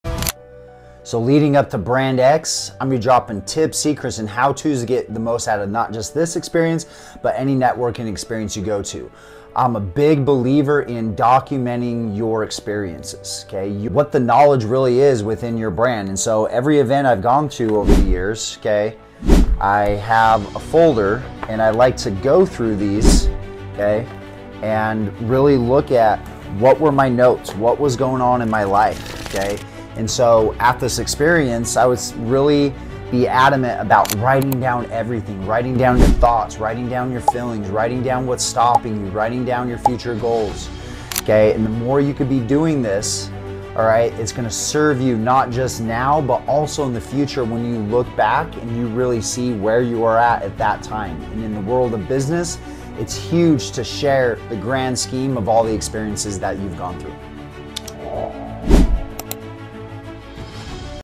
Brand-X is coming up on the sixth and seventh of September at the Phoenix convention Center.
We have some Speaker and vendor spots still available. To get tickets or apply go to brandxexpo.com

Want to build a Next Gen Brand that creates more Influence & Income?
Check out the FREE training "How to Evolve Your Brand to Create More Influence & Income"